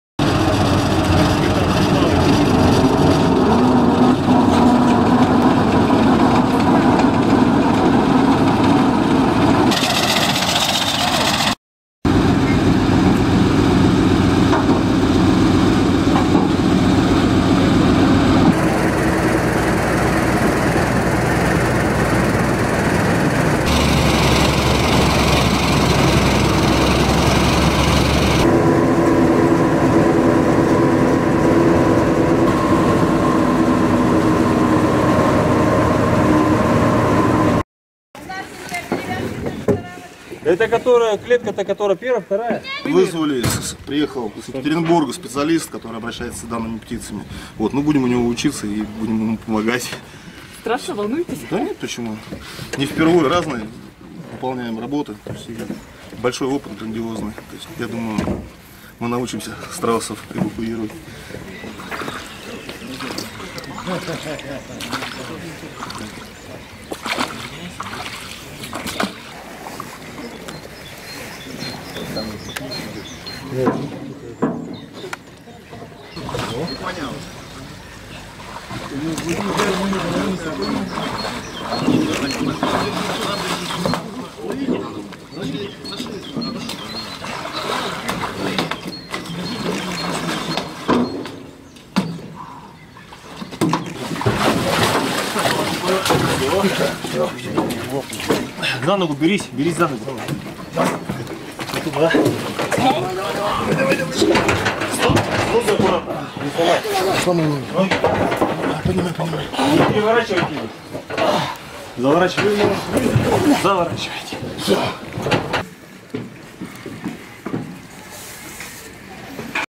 Спасение страусов от наводнения в Ирбите
источник - ГУ МЧС России по Свердловской области